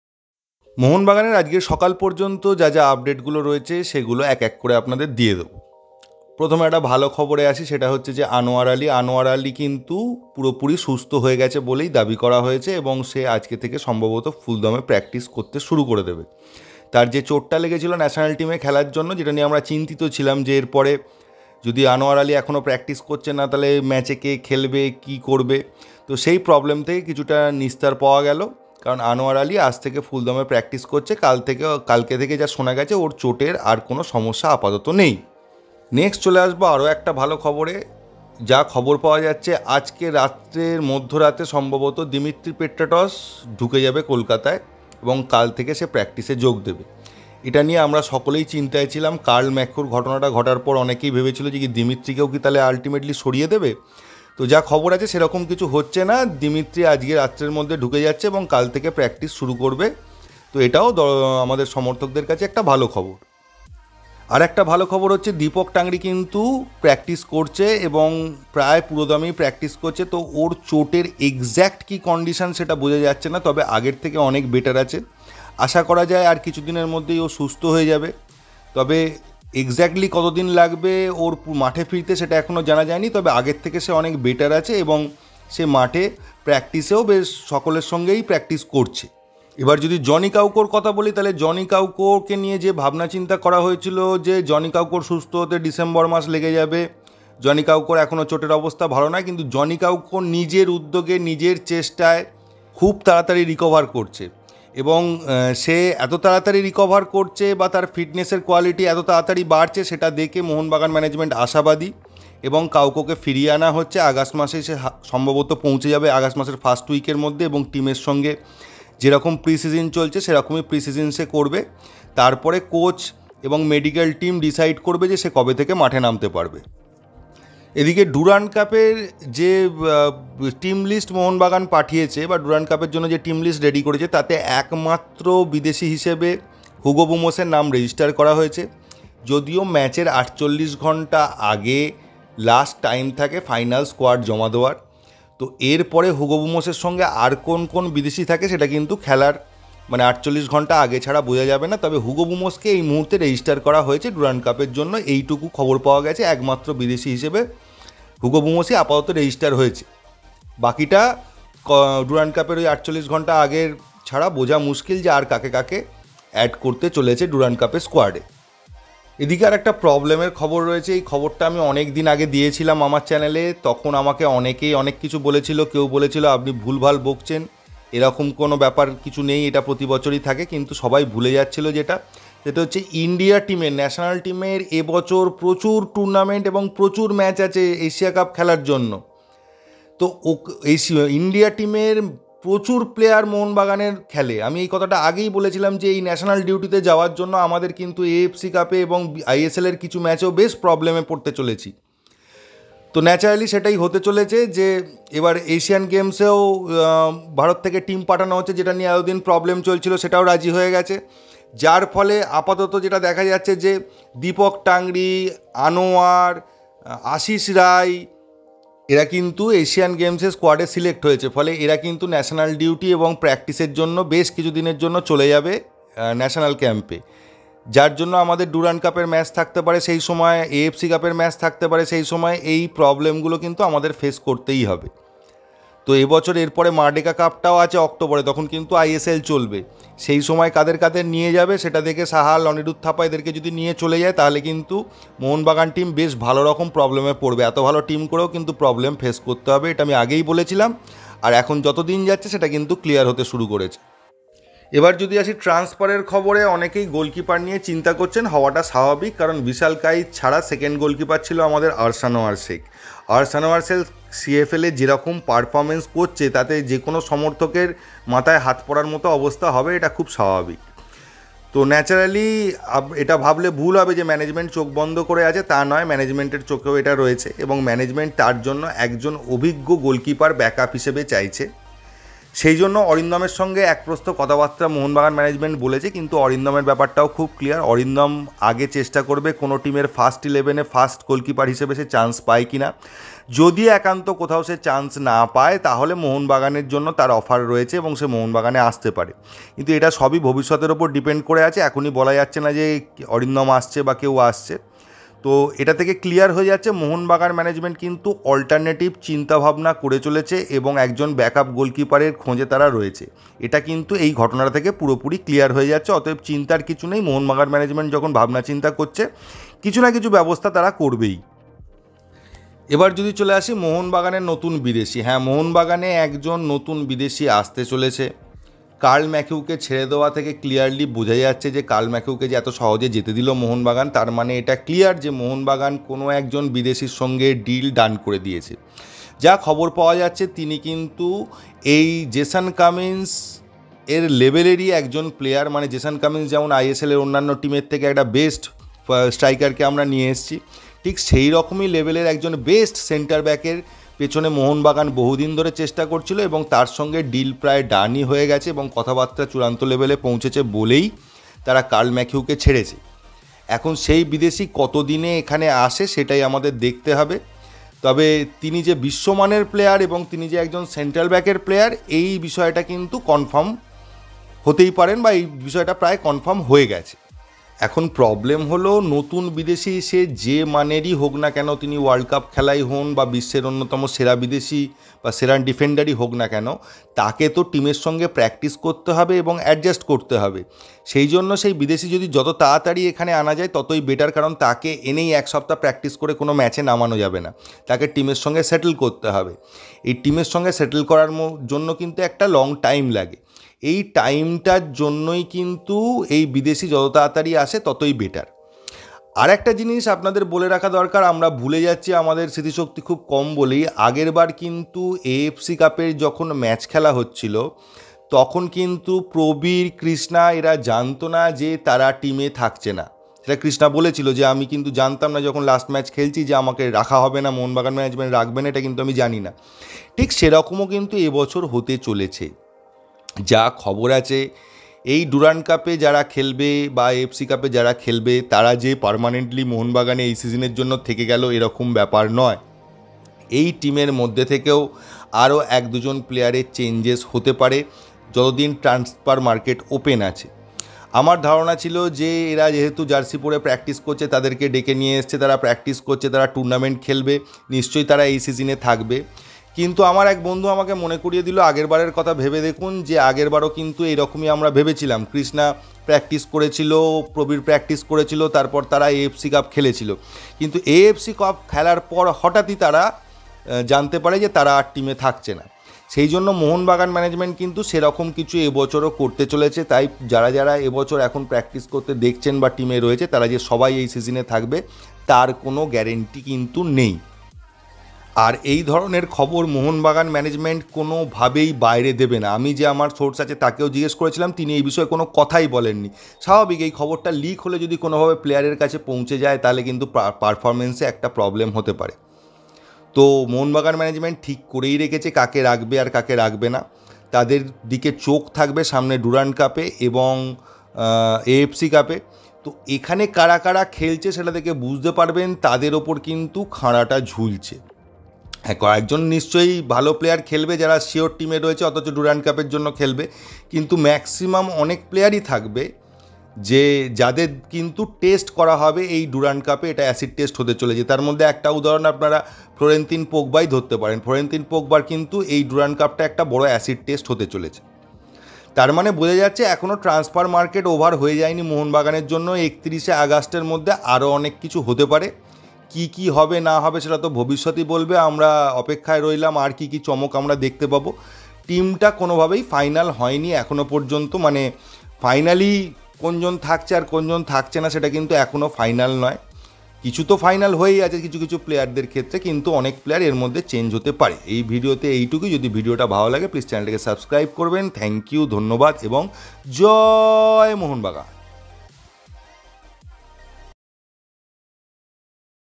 vishal এর replacement কি হবে? দিমি কবে আসবে, আনোয়ার কবে সুস্থ হবেmohunbagansupergiant
New Chanel for CFL